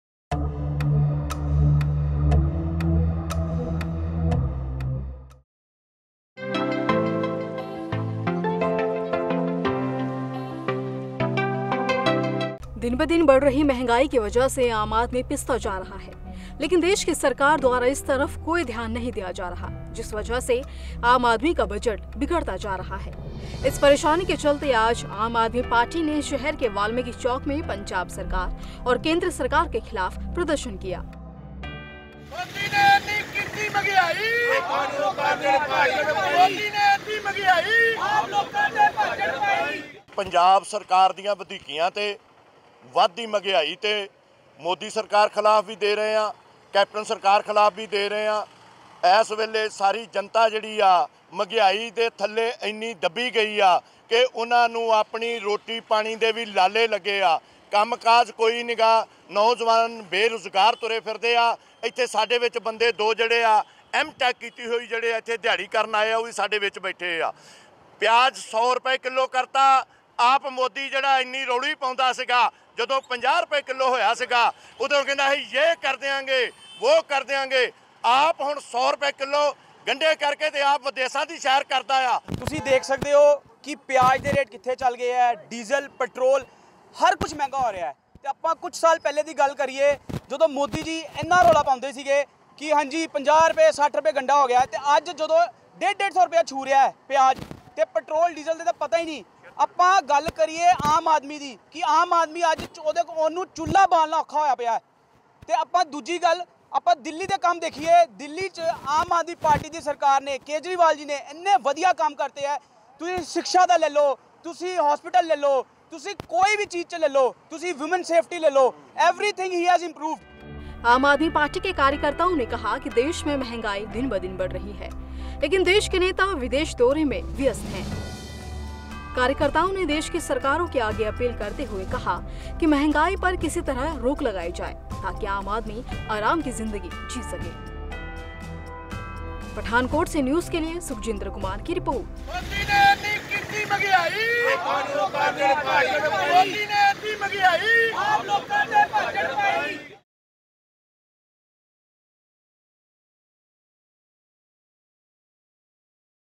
Pathankot: महंगाई के खिलाफ AAP का केंद्र और पंजाब सरकार के खिलाफ प्रदर्शन
दिन ब दिन बढ़ रही मंहगाई की वजह से आम आदमी पिस्ता जा रहा है लेकिन देश की सरकार द्वारा इस तरफ कोई ध्यान नहीं दिया जा रहा जिस वजह से आम आदमी का बजट बिगड़ता जा रहा है। इस परेशानी के चलते आज आम आदमी पार्टी ने शहर के वाल्मीकी चौक में पंजाब सरकार और केंद्र सरकार के खिलाफ प्रदर्शन किया